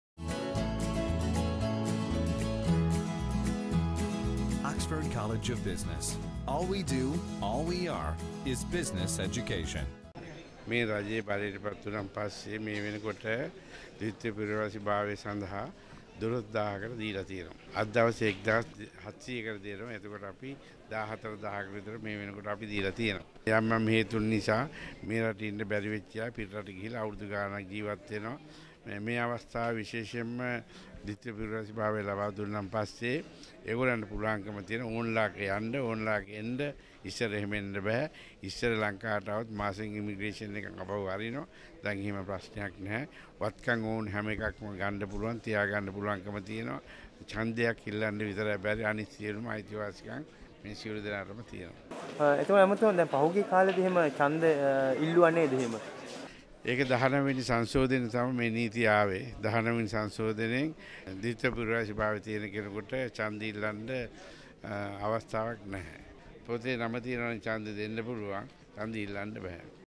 Dual citizens cannot contest Parliamentary Polls - Navinna
14,000ට මේවන විටcදී ඇති බව එස්.බී නාවින්න ඇමති කියයි.